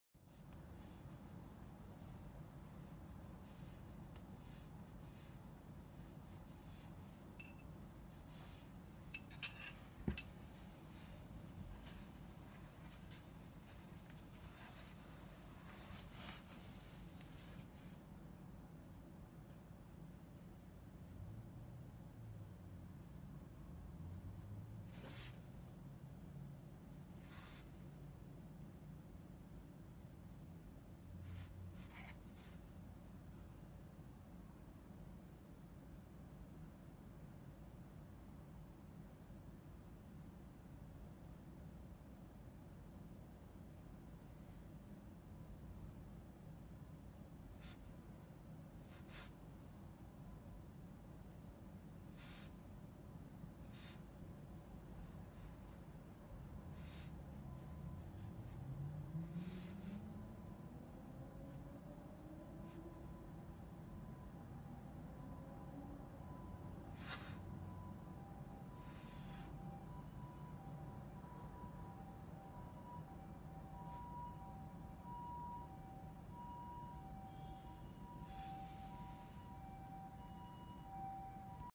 Drawing live＿2022.02.02より「出張」が、うまれました。
Happy Birthday Drawing その日の誕生日を祝って、気ままに落書きの実験をしています。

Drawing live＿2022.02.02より
「出張」が、うまれました。

『明日は朝から大忙しだな。』

使用画材:顔料スプーン／朱墨汁／筆ペン

1分22秒
1minutes and  22seconds
1menit dan  22detik

1997年　多摩美術大学院修了